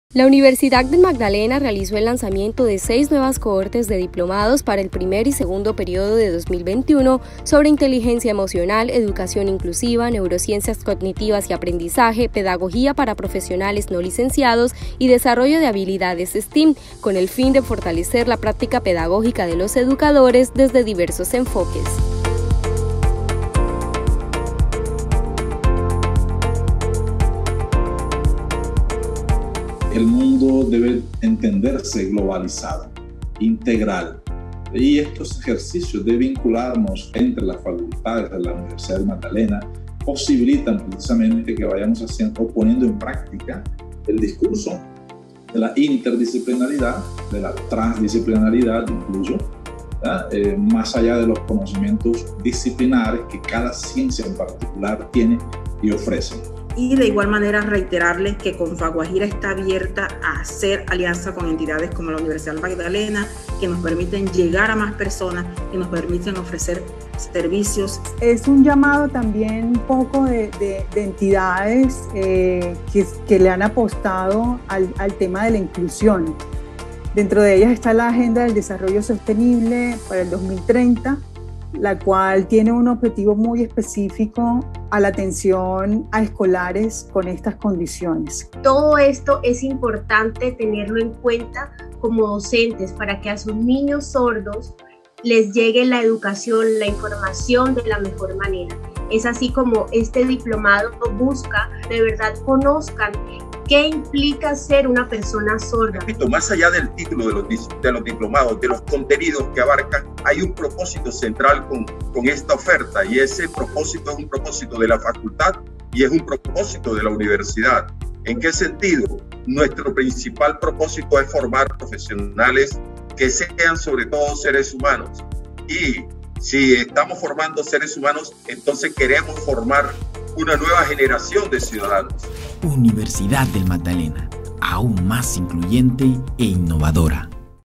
UNIMAGDALENA oferta seis diplomados para educación incluyente e innovadora
Con el fin de fortalecer los procesos de inclusión, desarrollo personal, innovación y liderazgo desde la práctica pedagógica, la Universidad del Magdalena dio apertura a seis nuevas cohortes de diplomados para el primer y segundo período de 2021, liderados por la Facultad de Ciencias de la Educación y el Centro de Postgrados y Formación Continua.

La oferta está dirigida a estudiantes, egresados y personal externo de la Alma Mater interesados en profundizar sobre inteligencia emocional, educación inclusiva, neurociencias cognitivas y aprendizaje, pedagogía para profesionales no licenciados y desarrollo de habilidades STEAM.